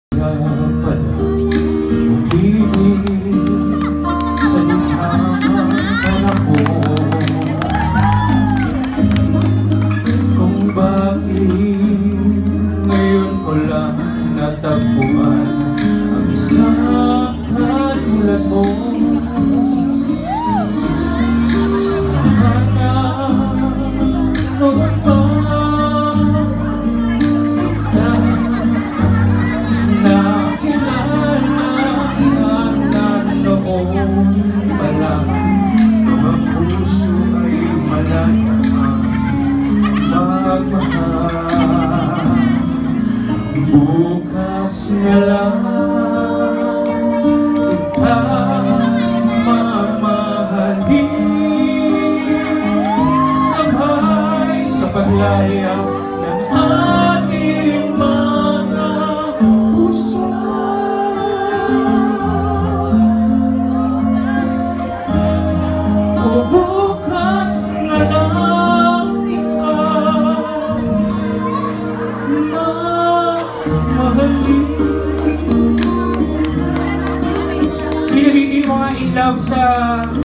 MARLON's performance at Miss OFW Wonder Woman Season 2.
MARLON's performance at Miss OFW Wonder Woman Season 2 held at Le Danz Club, Singapore last 28 April 2019.
Video by: KAP TV